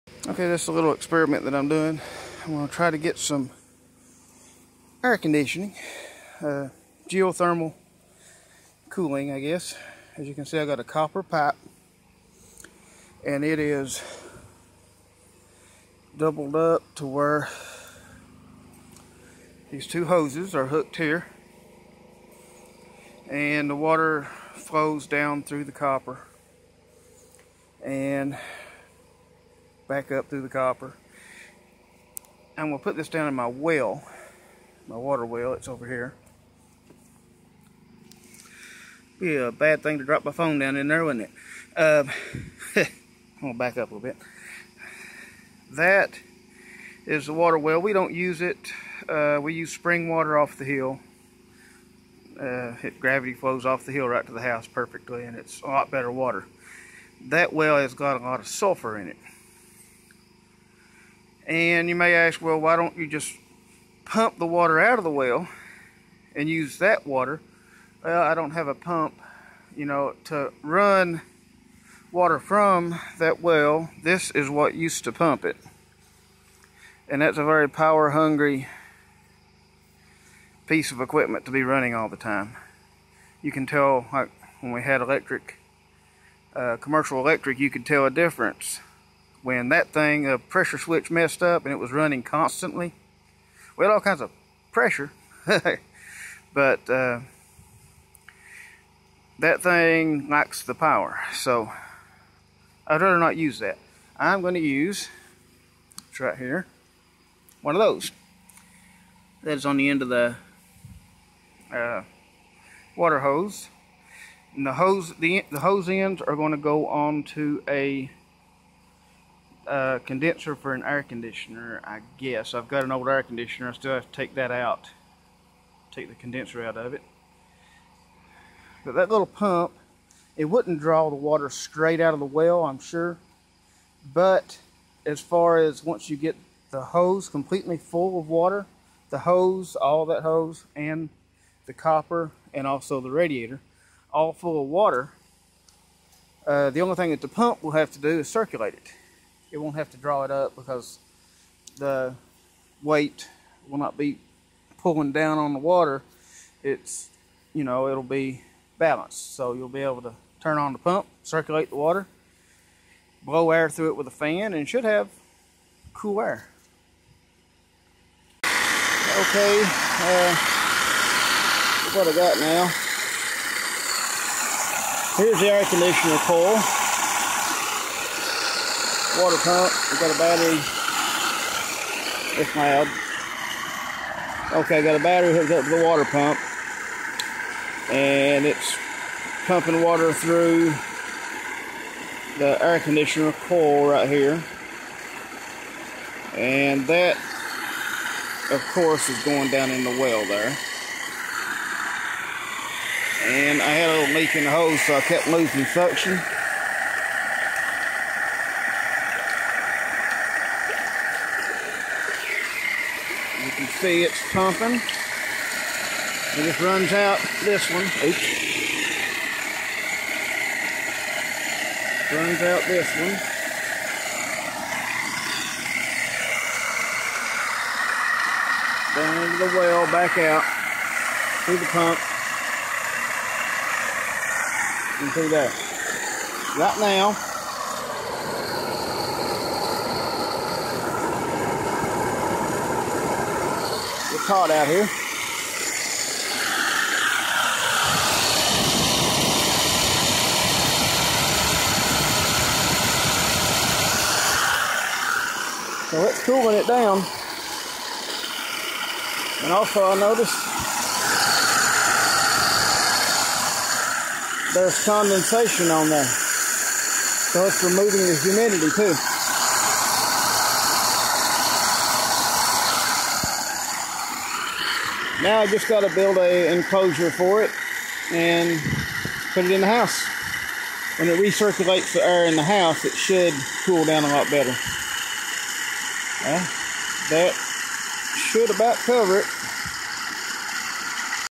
easy DIY geothermal cooling system.
I'm trying to figure out some low energy alternative way to have air conditioning.  My off grid power system can't keep up with a conventional unit so I figured I would try this.